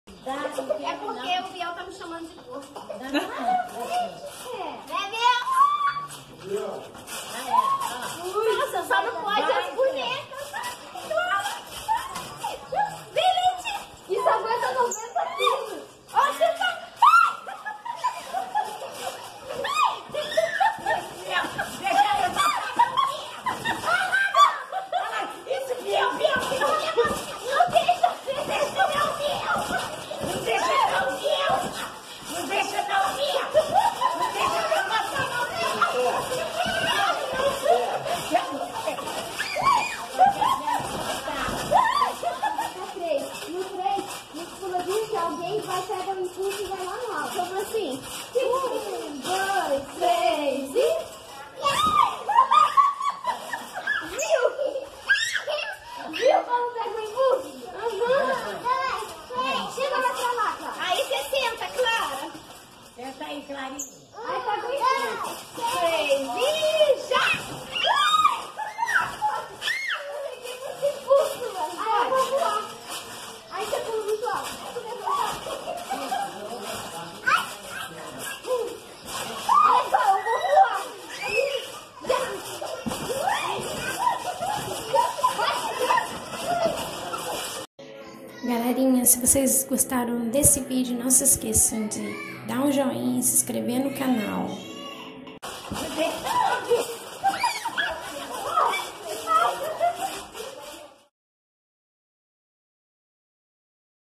Brincando no Pula-Pula com Mamãe/ Baby Alive e Bebê Reborn no pula pula/ Brincado com a mamãe
Nesse Natal de 2020 tem muita diversão no Pula- pula com a Mamãe e meu primo Gabriel
BRINCADEIRAS bem divertidas com as BONECAS Baby Alive e a Bebê Reborn
pegadinhas divertidas trolagens tudo com muita alegria em Família no Pula 😃 👍
Brincando no pula pula com a mamãe Diversão em familia

Espero que  gostem desses especial de Natal

Quer vídeos engraçados?
Deixe aí nos comentários 😃 qual vídeo você gostaria que eu fizesse 🥰